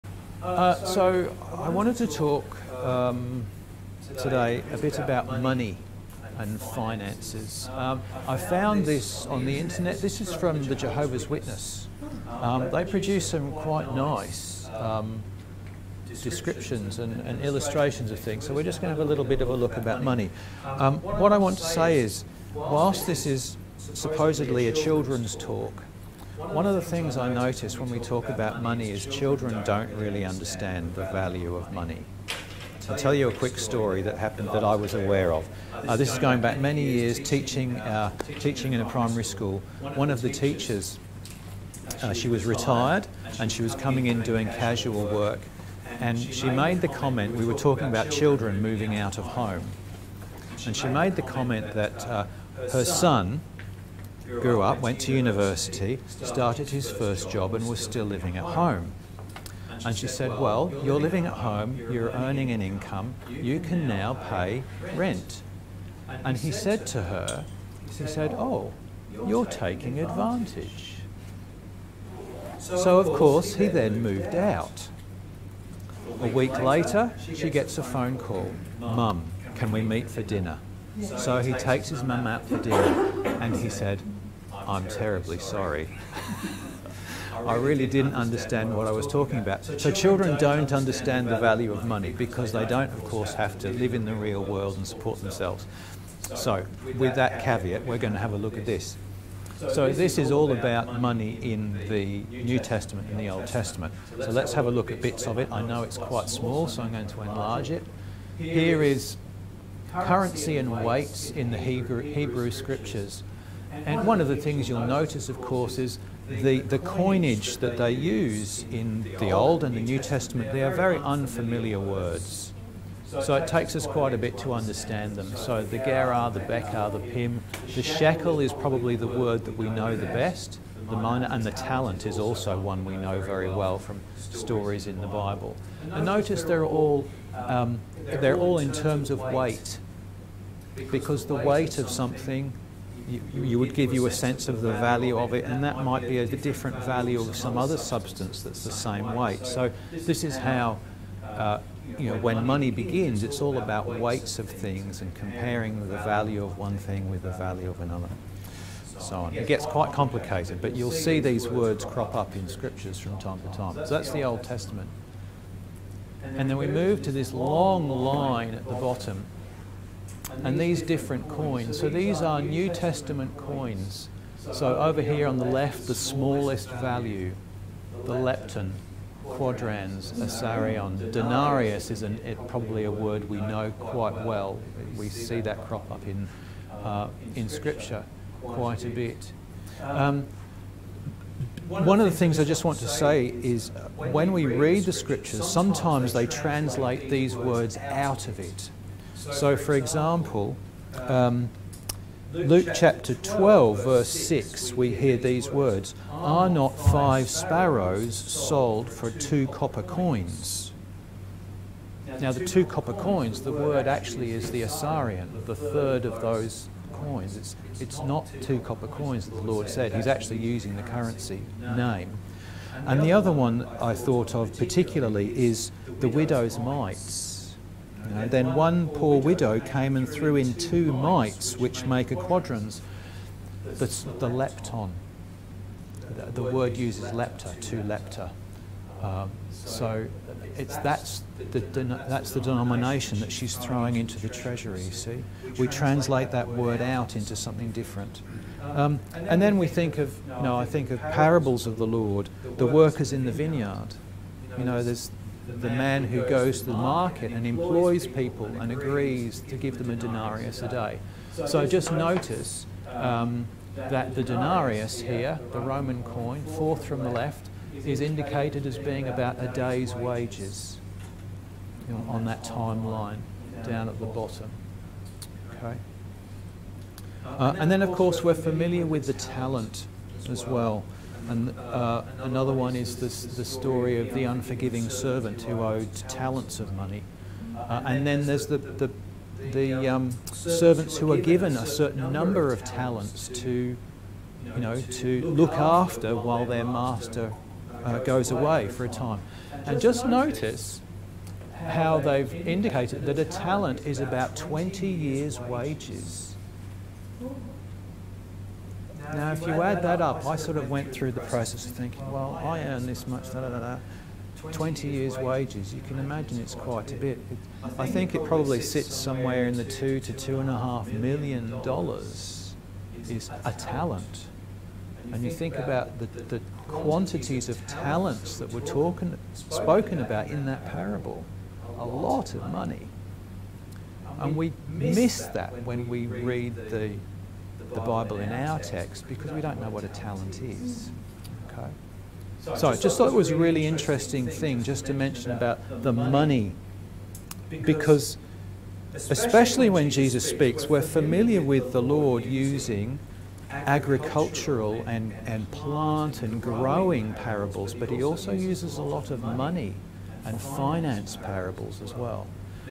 Money in the Bible
We explored the different coins and denominations referred to in the text of the Bible, as they're very different from the coins we're used to seeing in the modern world.

Our children's talk, from Sunday 17 September.